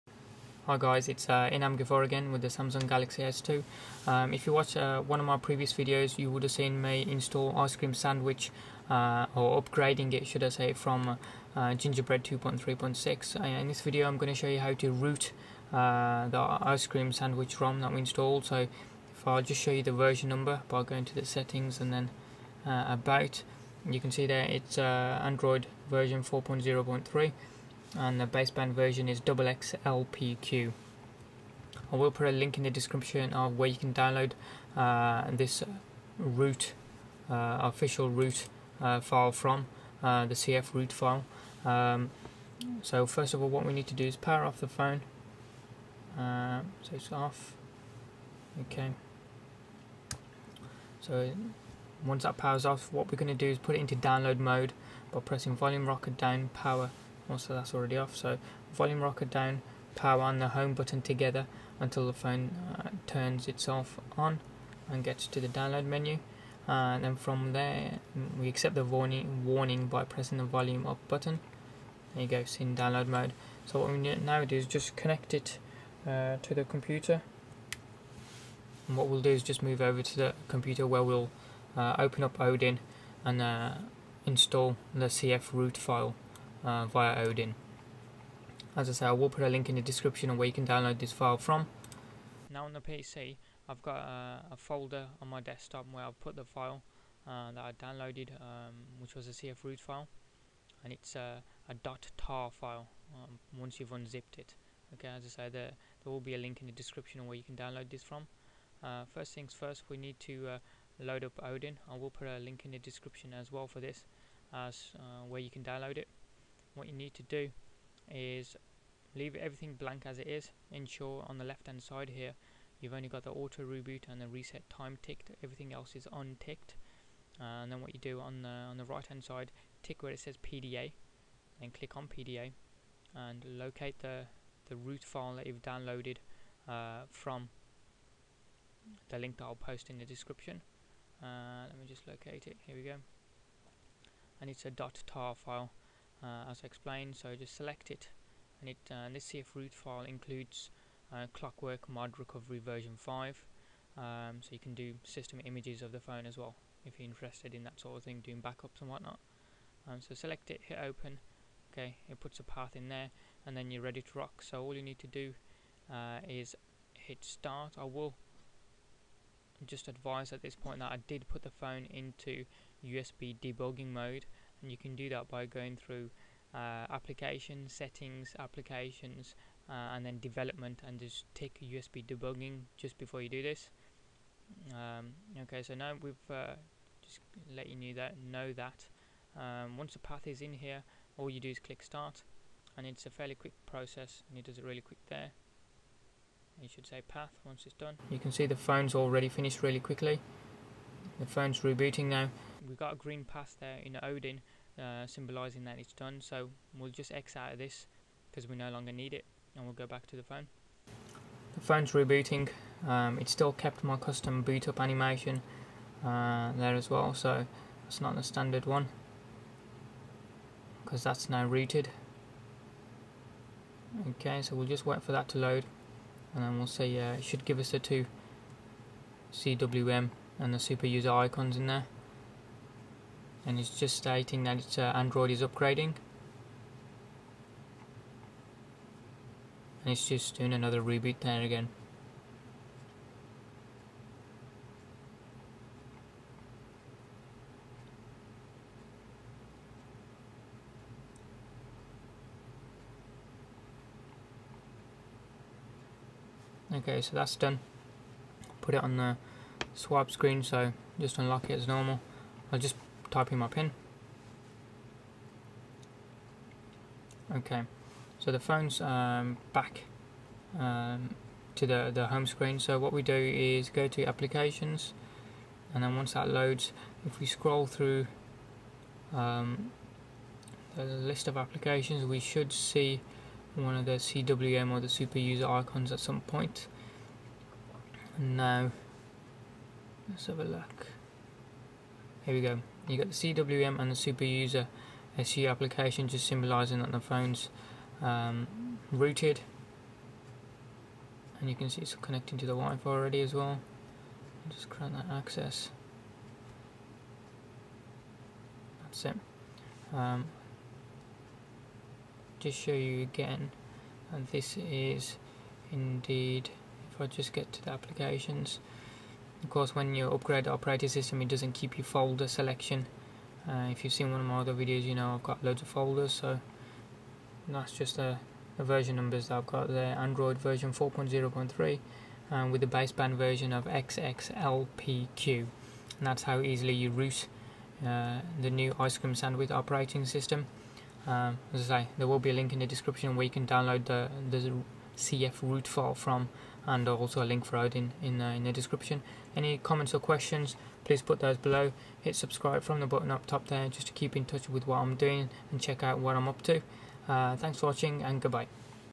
How to Root Official Ice Cream Sandwich (ICS) Released March 2012 Android 4.0.3 on Galaxy S2
How to root the newly released Official Ice Cream Sandwich (ICS) ROM which is Android 4.0.3 on a Samsung Galaxy S2 / SII, This Rom was Officially released by Samsung in March 2012 so check out how to give yourself Root Access.